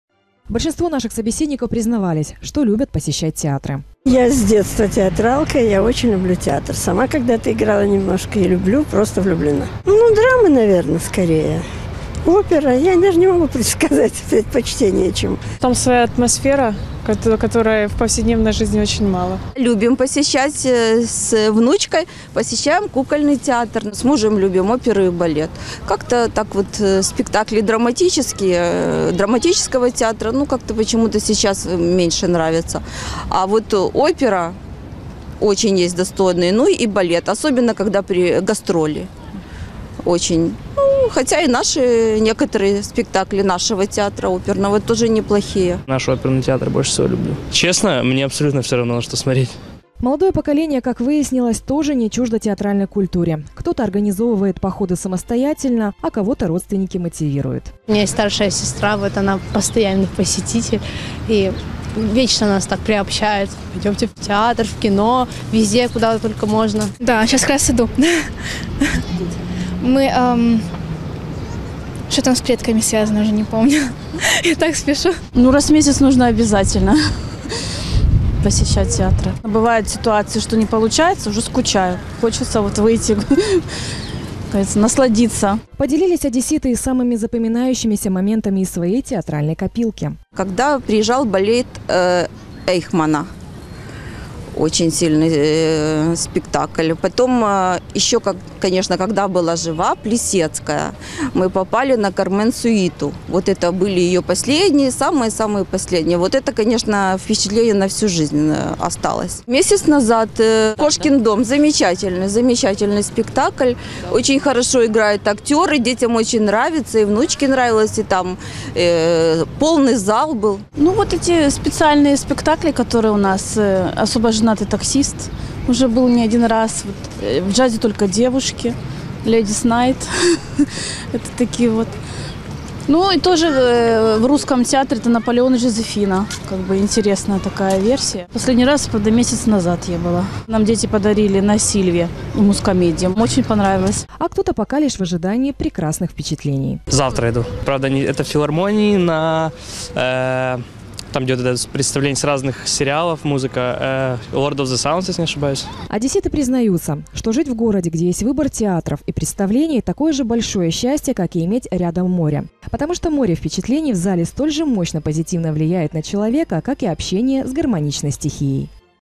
Одесситы - заядлые театралы
Одесса богата не только творческими людьми, которые дарят свое искусство публике, но и ценителями этого дара. В Международный день театра, наши корреспонденты ТРК «ГРАД» узнавали у горожан, как часто они выделяют время на посещение спектаклей и концертов.
Большинство наших собеседников признались, что любят посещать театры. Молодое поколение, как выяснилось, тоже не чуждо театральной культуре. Кто-то организовывает походы самостоятельно, а кого-то родственники мотивируют.
Одесситы признаются, что жить в городе, где есть выбор театров и представлений, такое же большое счастье, как и иметь рядом море. Потому что море впечатлений в зрительном зале столь же мощно позитивно влияет на человека, как и общение с гармоничной стихией